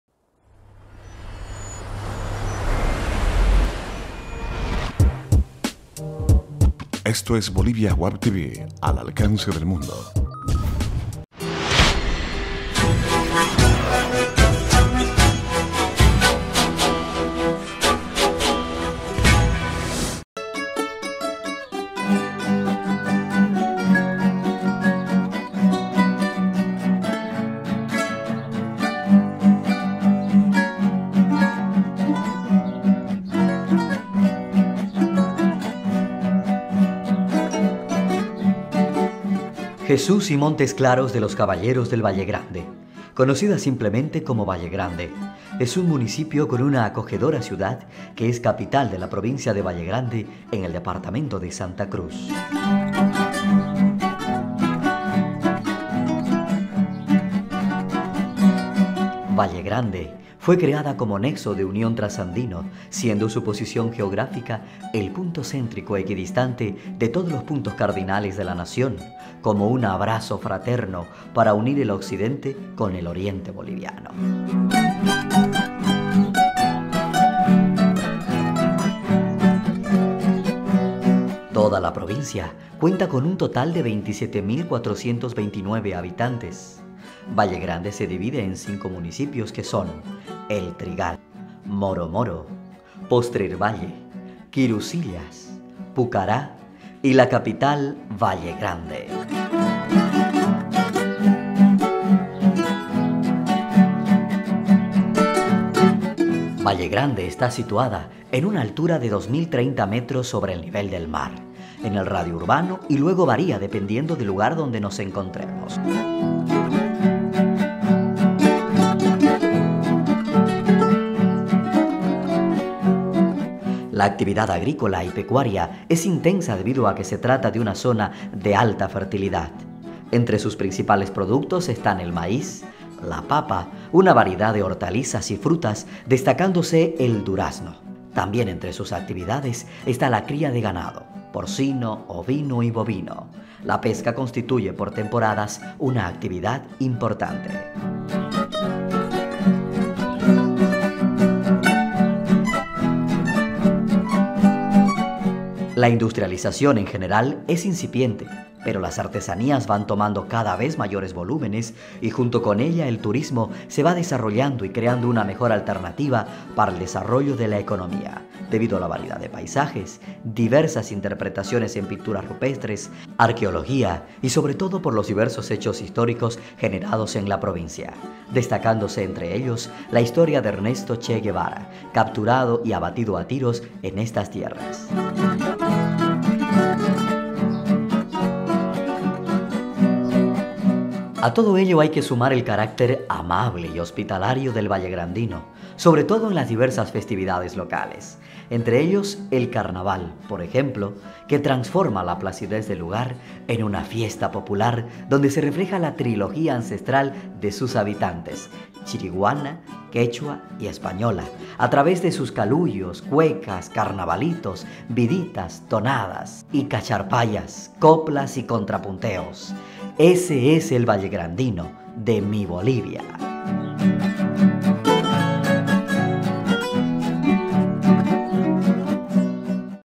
SOY BOLIVIA - VALLEGRANDE
- Jesús y Montes Claros de los Caballeros del Vallegrande, fue fundada 30 de marzo del año 1612 por mandato del Virrey del Perú Don Juan de Mendoza y Luna Marqués de Montes Claros el cual le fue encomendado a su sobrino,  conocida simplemente como Vallegrande, Toda la provincia cuenta con un total 27.429 habitantes. Vallegrande se divide en cinco municipios, que son: El Trigal, Moro Moro, Postrervalle, Quirusillas, Pucará y la capital Vallegrande,